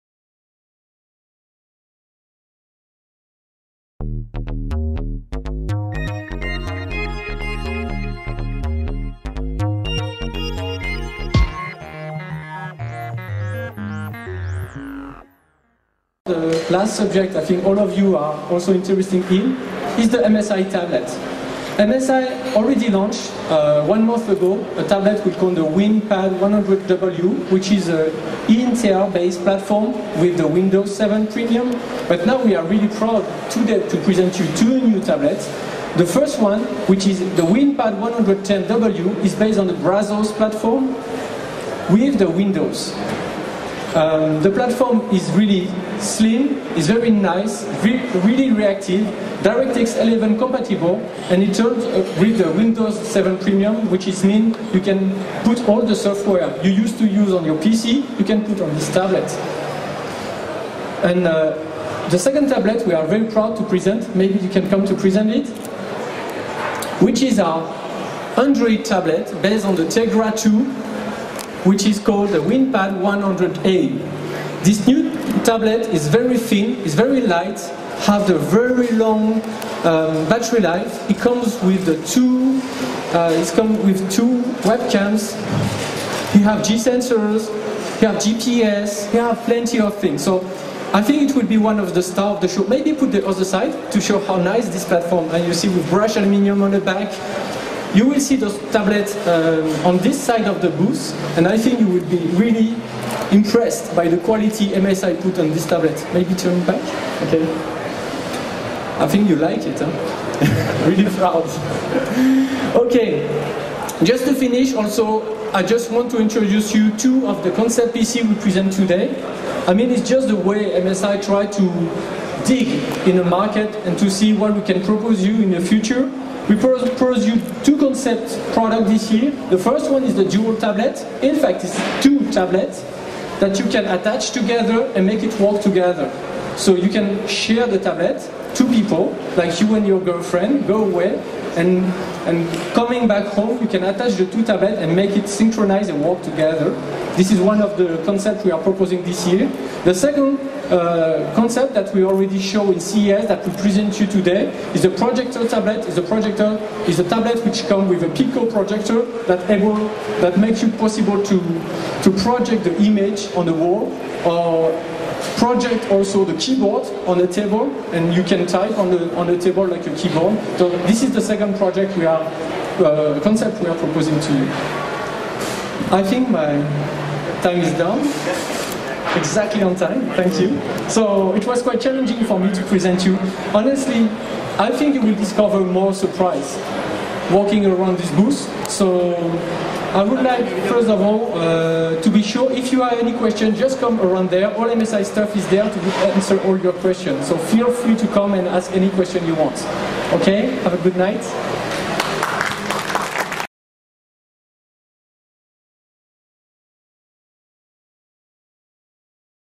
MSI DualPad, Projector Tablet and WindPads at CeBit
http //tabletblog.de - MSI Dual Pad and Projector Tablet at MSI press conference at CeBit 2011 - also MSI WindPad 110W, 100A and 100W Android 3.0 Honeycomb and Windows 7 Tablets